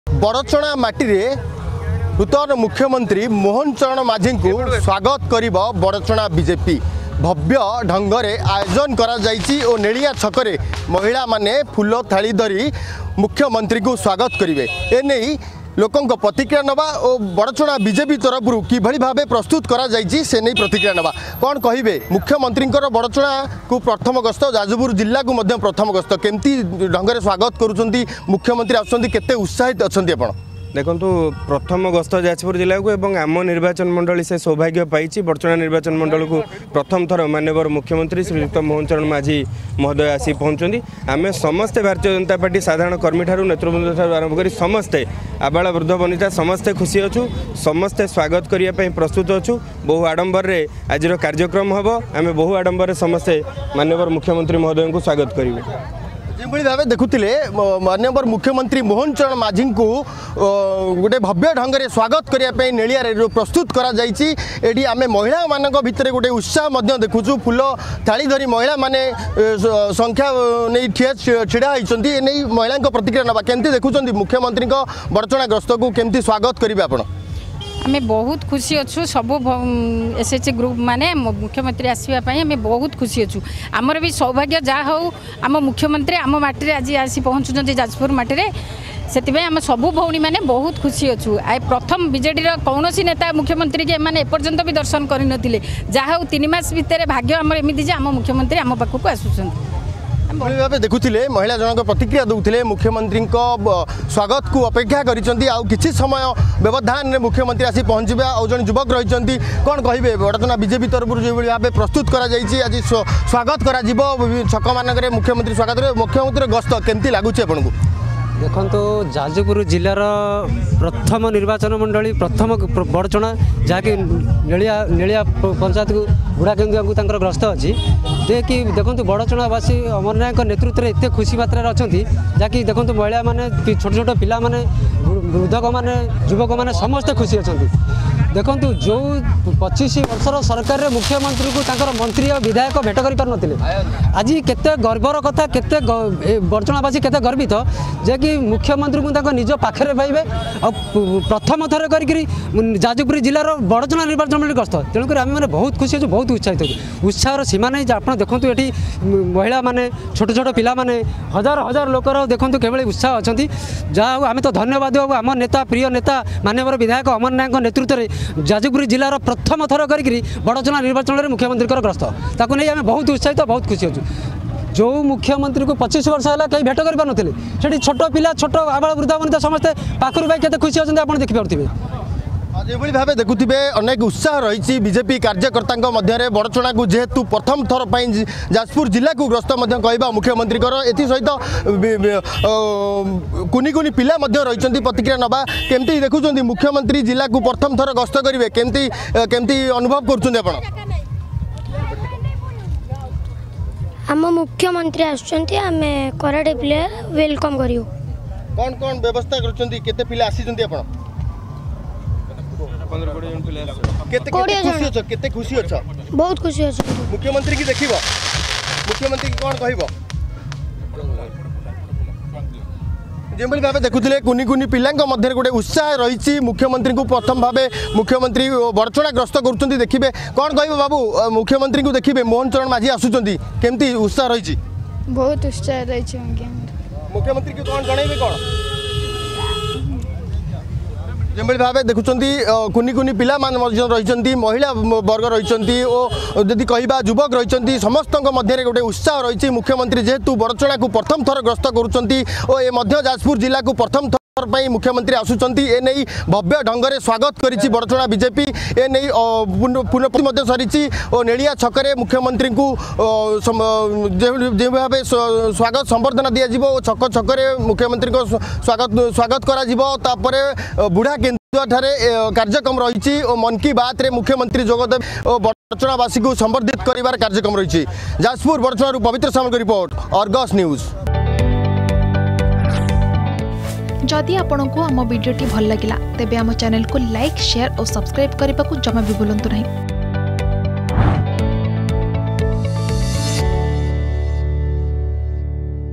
ମୁଖ୍ୟମନ୍ତ୍ରୀଙ୍କ ଗସ୍ତକୁ ନେଇ ବଡ଼ଚଣା ବିଜେପି ପକ୍ଷରୁ ବିରାଟ ଆୟୋଜନ | arrangements by the BJP for CM visit
ବଡ଼ଚଣା ନିର୍ବାଚନ ମଣ୍ଡଳୀକୁ ମୁଖ୍ୟମନ୍ତ୍ରୀଙ୍କ ପ୍ରଥମ ଗ୍ରସ୍ତକୁ ନେଇ ଜୋରଦାର ପ୍ରସ୍ତୁତି କରିଛି ବଡ଼ଚଣା ବିଜେପି। ଆଜି ଯାଜପୁର ଜିଲ୍ଲକୁ ପ୍ରଥମ ଥର ପାଇଁ ମୁଖ୍ୟମନ୍ତ୍ରୀ ମୋହନ ଚରଣ ମାଝୀଙ୍କ ଗ୍ରସ୍ତ କାର୍ଯ୍ୟକ୍ରମ ରହିଛି।